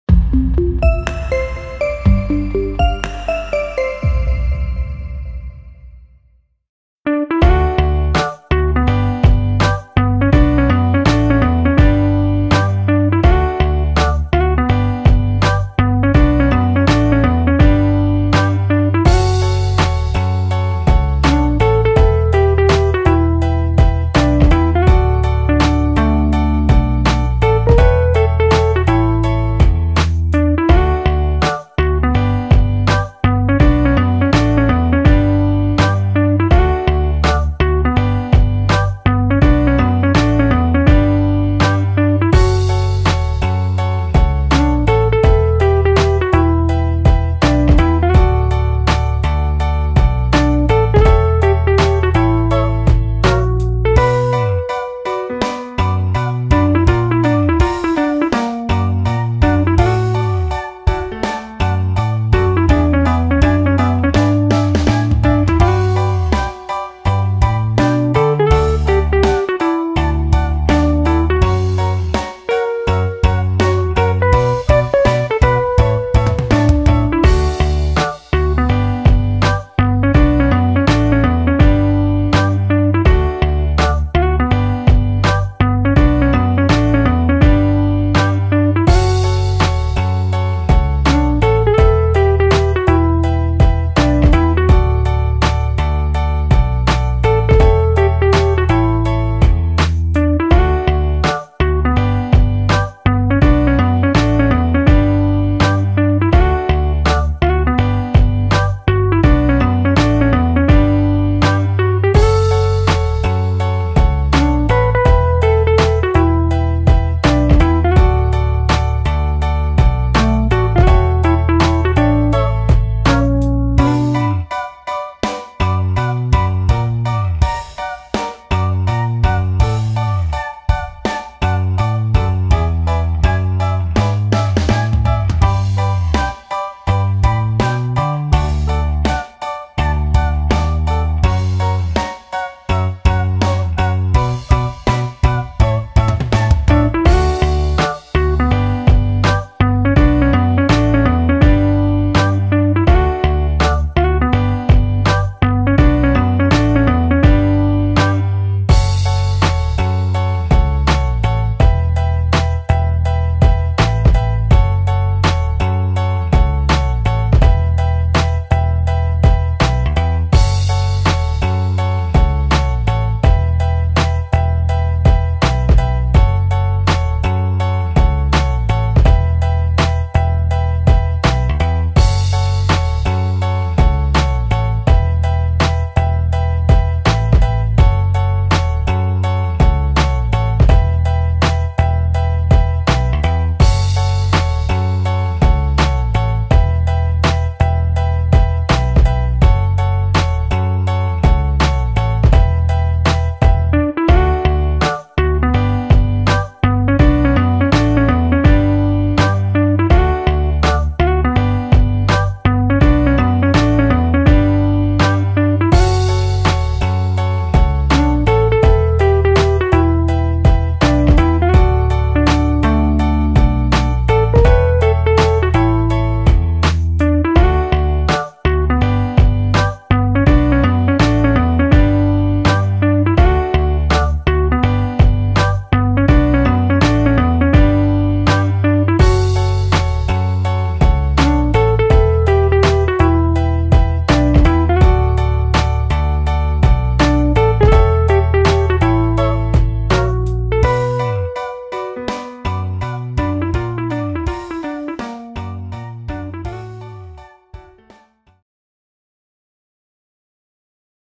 Wow! Ice cream! Look at how charmingly it melts! Timelapse!
Today, we have a special video: we'll show you how ice cream melts in time-lapse mode. Discover the magic of slow melting of this delicious treat.